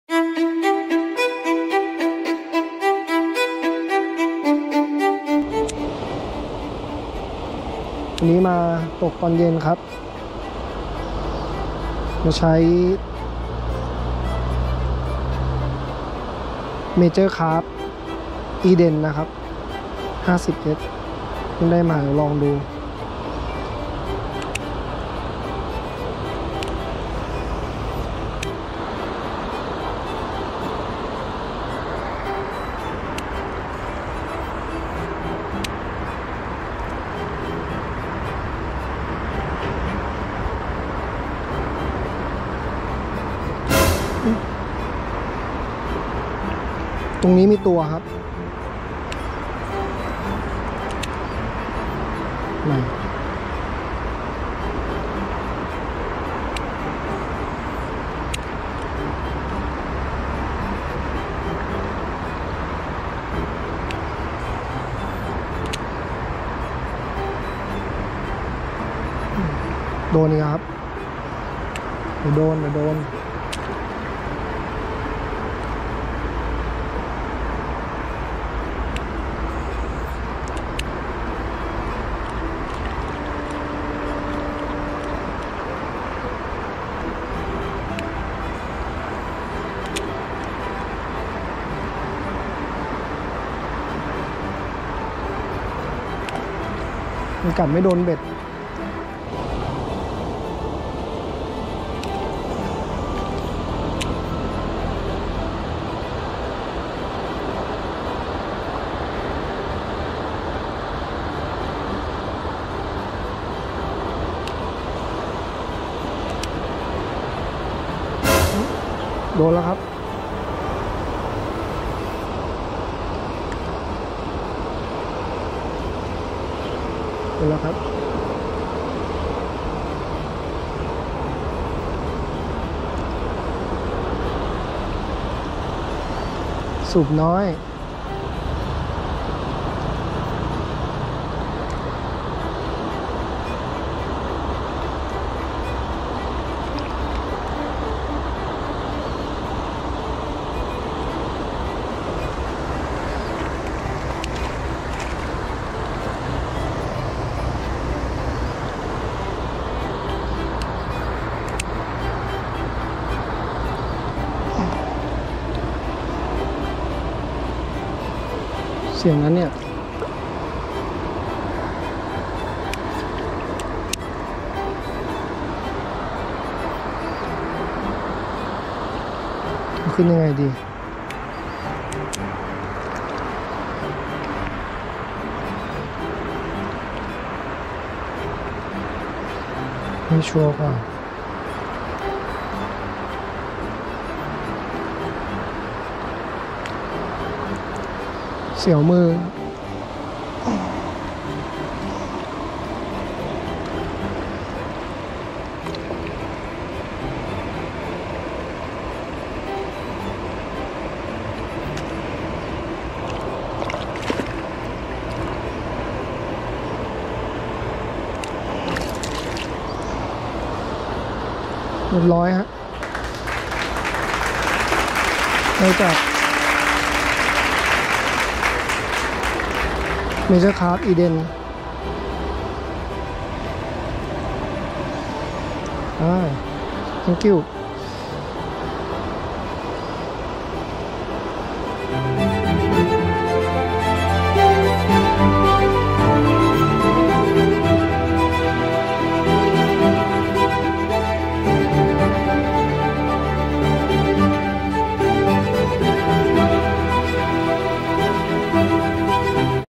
Major Craft 🍏Eden 5s | อืม…ก็okอยู่นะ👌🏻
16/11/65
UL Stream @ประตูน้ำ จ.พิษณุโลก
Rod: Shimano Cardiff Native Special B42UL-3 2-6lb 3ท่อน
Reel: Shimano CQ51 Avail Tune
Lure: Major Craft Eden 50s
PE: YGK G-Soul Egi-Metal X4 PE0.5
Shock Leader: Okawa Steel 10lb

วันนี้เป็นวันอะไรที่ผมเซ็งมากๆ เพราะงานที่ผมวางแผนเอาไว้มันกลับไม่เป็นอย่างที่คิด

ผมเลยถือโอกาสมาคลายเครียดสักหน่อย กับหมายเดิมๆแถวบ้านในช่วงเย็น โดยเหยื่อที่ผมเลือกใช้ในทริปนี้ ก็คือเจ้าEden 50s ของMajor Craft ที่ผมพึ่งซื้อเข้ามา ผมเล็งเอาไว้นานแล้ว เพราะเคยเห็นในคลิปของโปรยุ่น แต่ยังหาราคามือ2ไม่ได้ ผมว่ามันสวยดี หน้าตาเหมือนปลาTrout

ผลงานของมันจะเป็นอย่างไร ลองชมดูครับ (ไม่แห้วก็บุญแล้ว555)